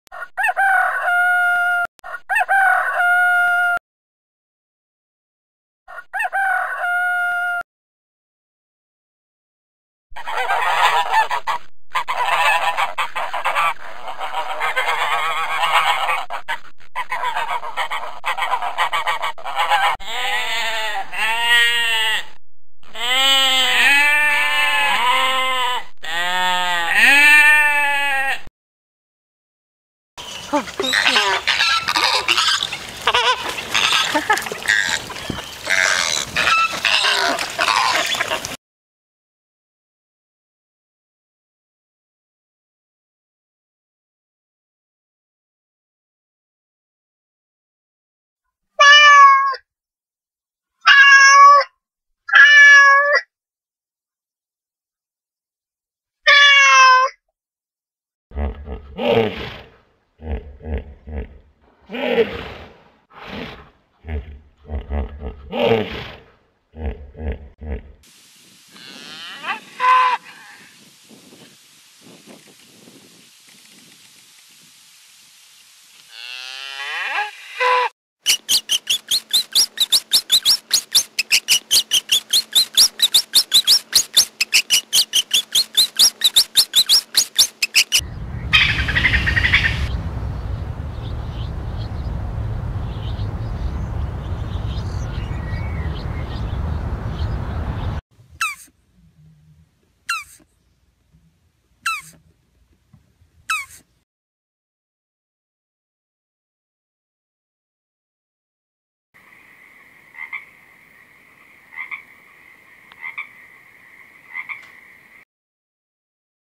What about this Animal's Sound!! Nice animal sounds (Rooster, Turtle, cat, Hippo, Rat, Fog ...)
Didi you gear turtle sound before?!! Nice Animal sounds for many animals.
Rooster sound
Turtle sound
cat sound Meow
Hippo sound
Rat sound
Fog sound